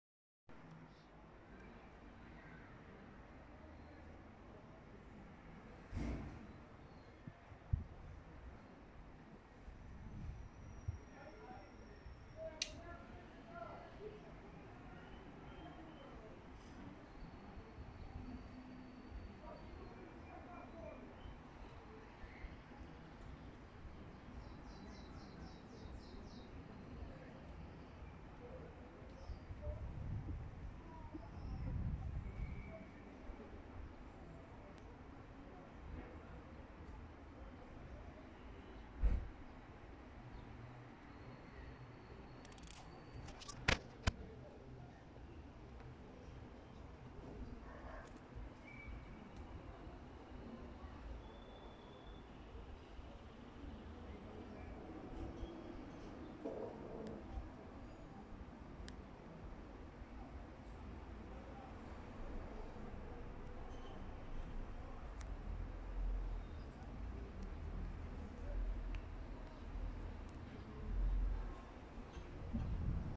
YOU CAN SEE THE HEART OF THIS TRANSPARENT SNAIL
This snails is so small and its shell so transparent that you can see his heart beat